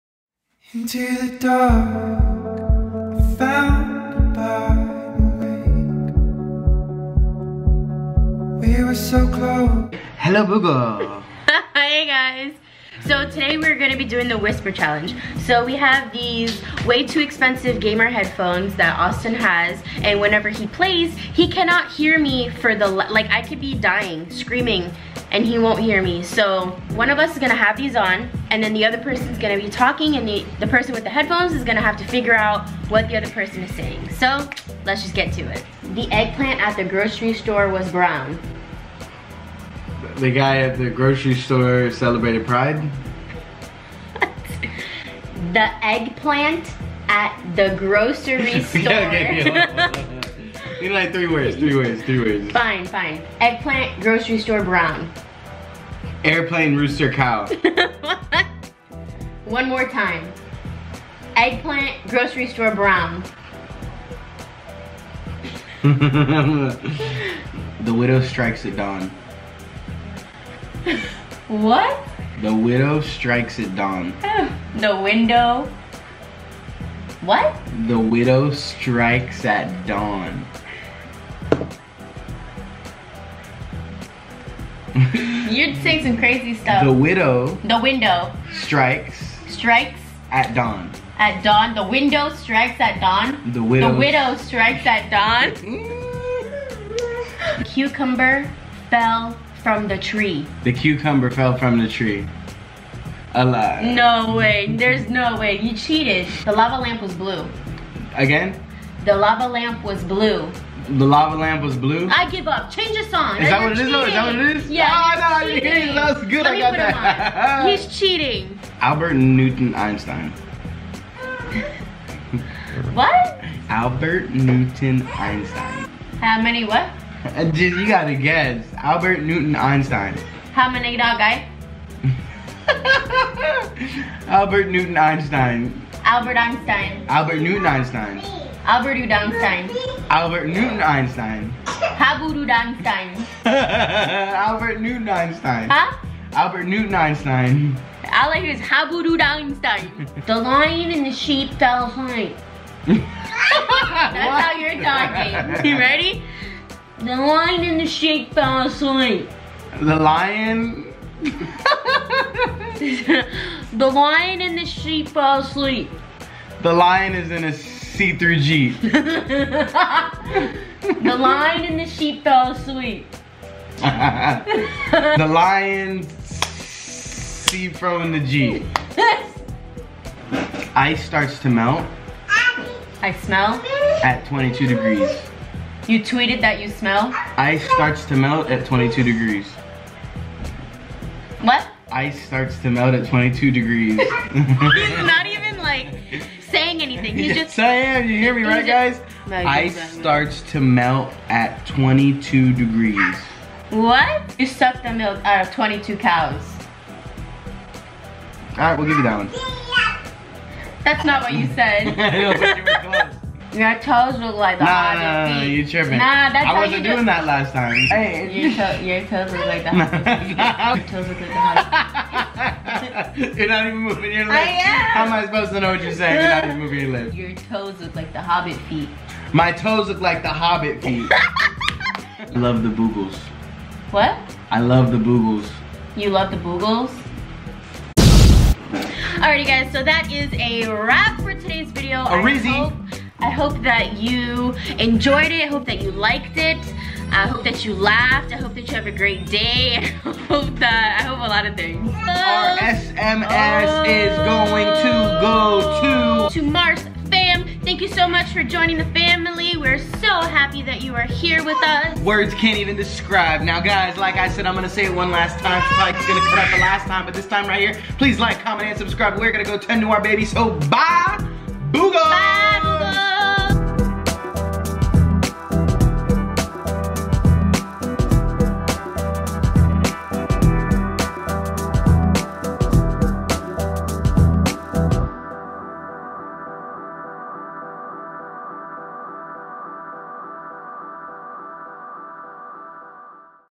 The Whisper Challenge! *funniest reaction*
We tried the whisper challenge today! Clearly Jessica needs to work on her hearing! Let us know what you think of the challenge!